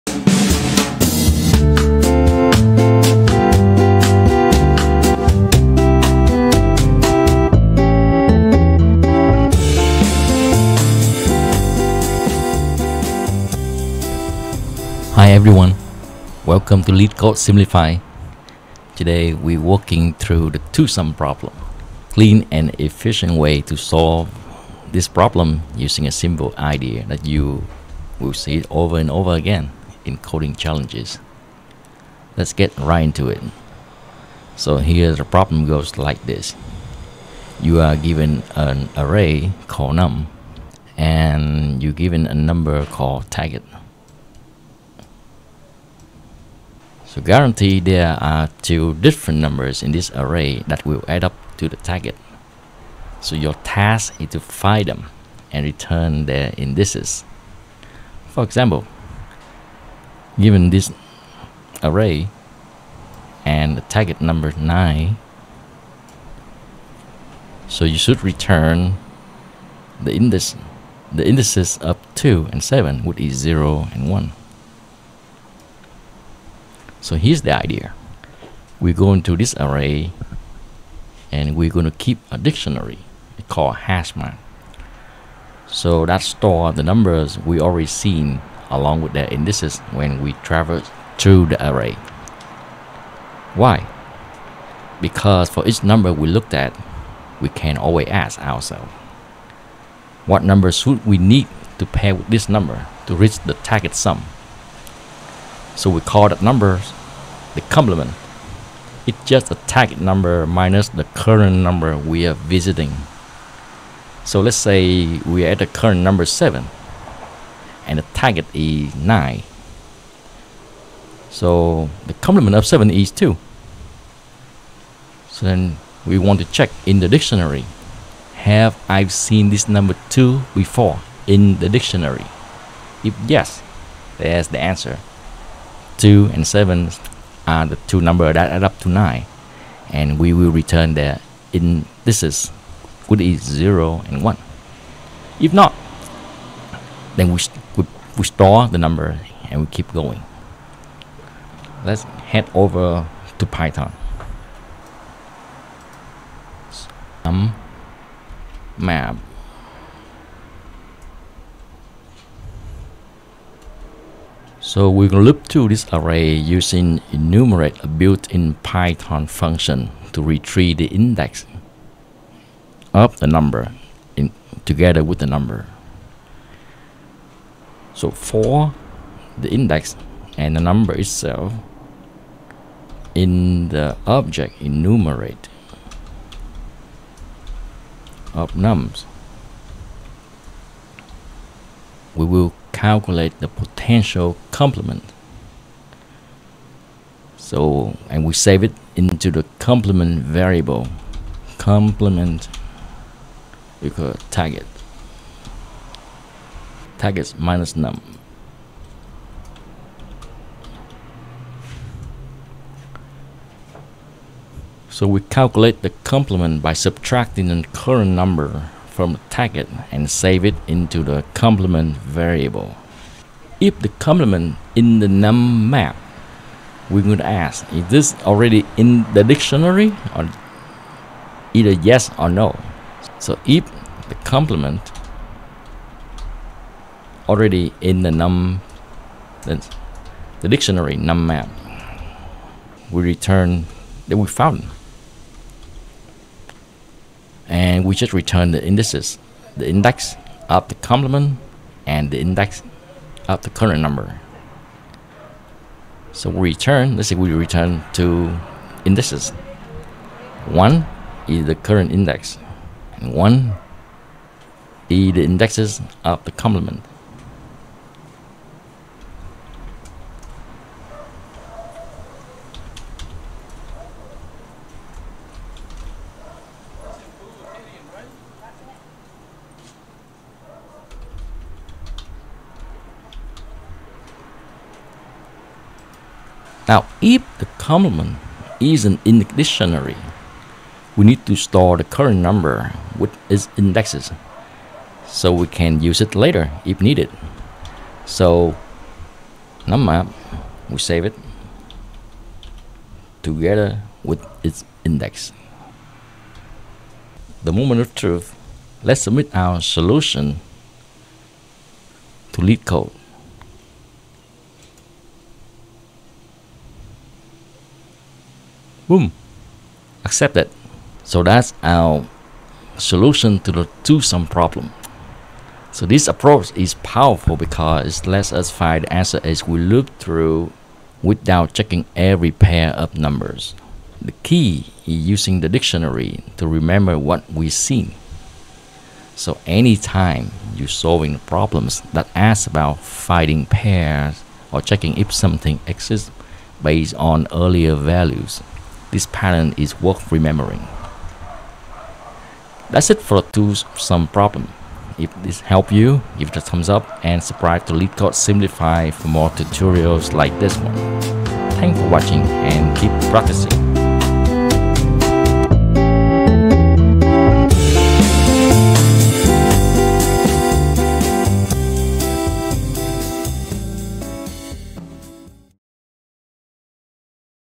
Leetcode 1. Two Sum ⚡ HashMap, Instant Pair Finder
We walk through the Two Sum problem from LeetCode using the cleanest and most efficient method.
If you’re preparing for coding interviews or brushing up on Python, this step-by-step breakdown will help you master the core idea behind this classic problem.